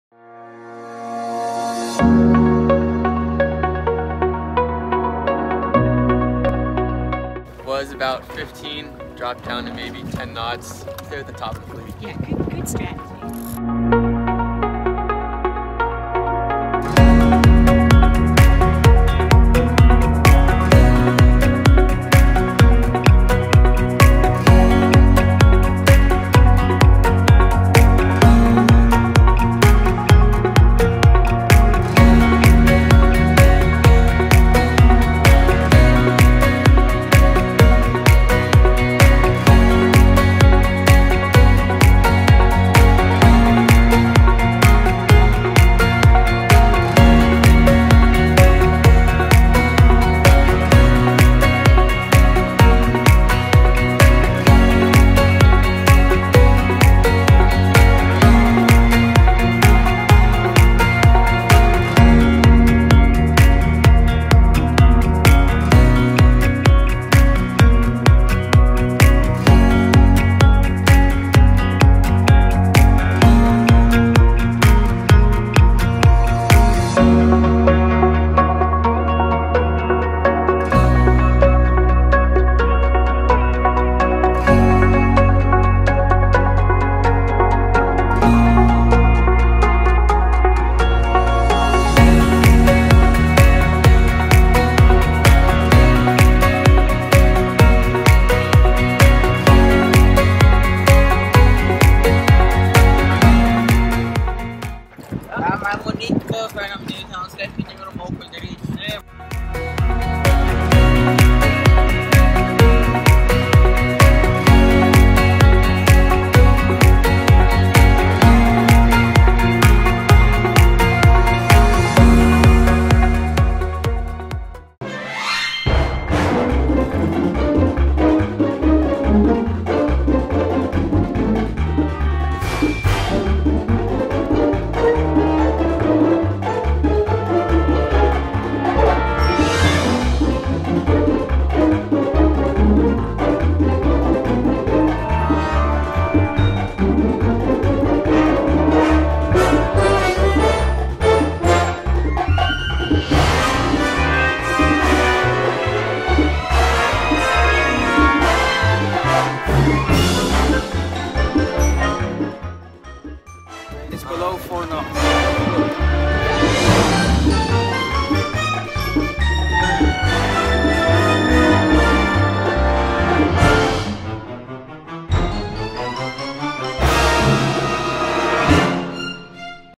Coaching Lasers at the Orange Bowl Regatta 2022
Coached the United Nations Team at the 2022 Orange Bowl International Youth Regatta in Miami.